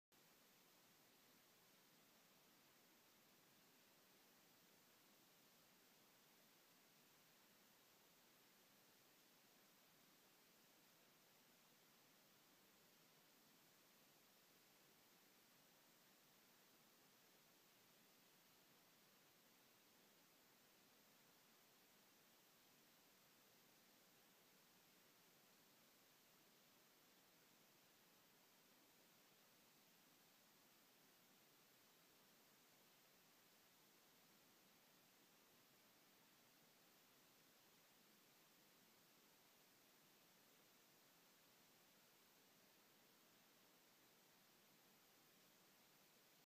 Cat attacks German shepherd
Cat doesn't back down to big Kane